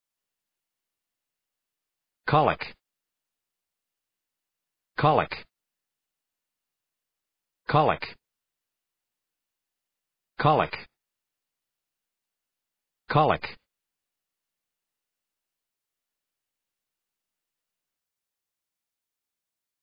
How to pronounce colic
colic American English pronunciation. How to pronounce colic correctly. How to say colic in proper American English.